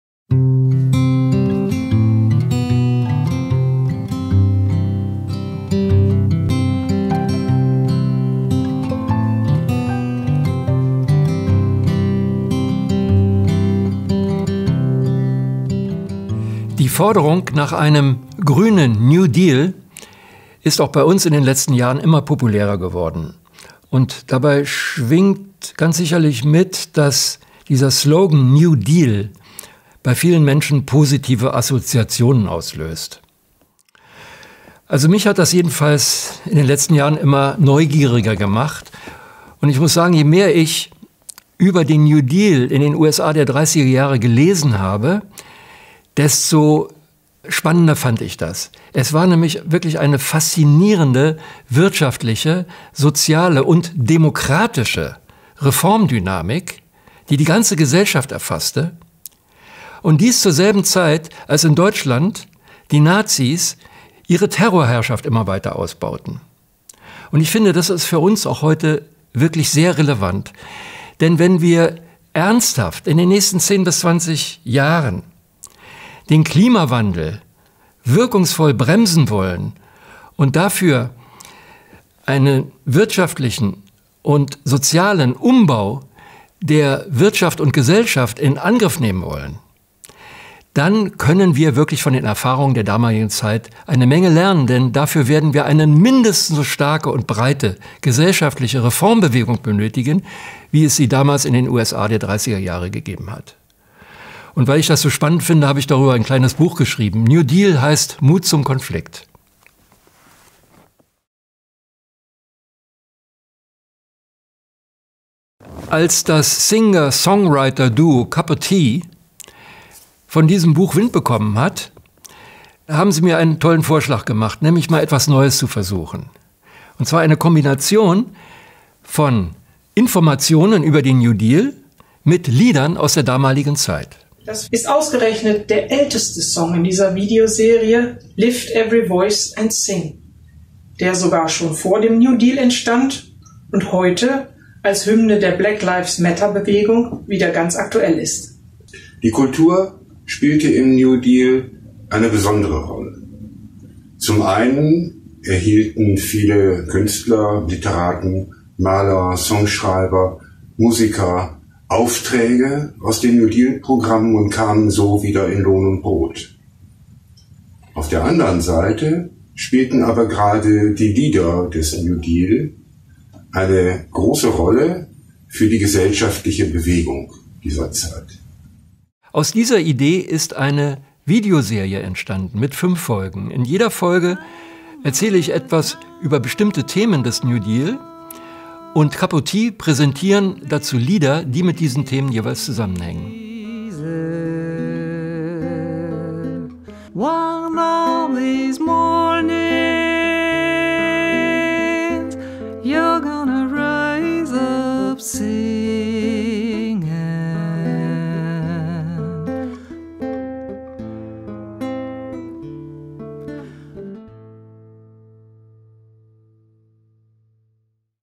Rise up Singin‘ – Wie der New Deal heute wieder Mut macht (Trailer)
⇧ Musikalisch-politische Reihe mit Cuppatea und Steffen Lehndorff ⇧

Ein Land in einer existenziellen Krise.
Ein Land im Aufbruch.
Eine Gesellschaft, die sich verändern will – und den Streit um den richtigen Weg nicht scheut.

Das waren die USA während der Weltwirtschaftskrise. Während in Deutschland die Nazis nach der Macht griffen, startete die Roosevelt-Regierung in den USA ein riesiges wirtschaftliches und soziales Aufbau- und Reformprogramm demokratischer Prägung. Viele, die sich heute für einen Green New Deal für den Klimaschutz engagieren, fühlen sich durch dieses historische Beispiel inspiriert.

Vom 7. bis 11. Juni 2021 wird auf diesem Kanal täglich eine neue Folge der Videoserie «Rise Up Singin'» veröffentlicht. Steffen Lehndorff berichtet über den demokratischen Aufbruch im New Deal, und das Cuppatea-Duo vermittelt musikalisch die emotionale Stimmung dieser Jahre.

⇗ Folge 1: Krise, Depression und neue Hoffnung – Premiere: 7. Juni 2021, 19 Uhr
    Link: youtu.be/_d9O1FewmQ4

⇗ Folge 2: Demokratischer und sozialer Aufbruch – Premiere: 8. Juni 2021, 19 Uhr
    Link: youtu.be/NDsuip5r1HI

⇗ Folge 3: Rassismus und Antirassismus – Premiere: 9. Juni 2021, 19 Uhr
    Link: youtu.be/Qo5wMCA5M-k

⇗ Folge 4: Sozialreformen und Gewerkschaften – Premiere: 10. Juni 2021, 19 Uhr
    Link: youtu.be/ZA6e8OMjK3o

⇗ Folge 5: Mut zum politischen Konflikt – Premiere: 11. Juni 2021, 19 Uhr
    Link: youtu.be/MMHjpWqyBWM

Zu den Premieren jeweils um 19 Uhr wird Cuppatea im Live-Chat dabei sein und Euch für Fragen, Kommentare und Austausch zur Verfügung stehen.

㊂ Alle Videos sind untertitelt.

Steffen Lehndorff (2020): «New Deal heißt Mut zum Konflikt», VSA Verlag.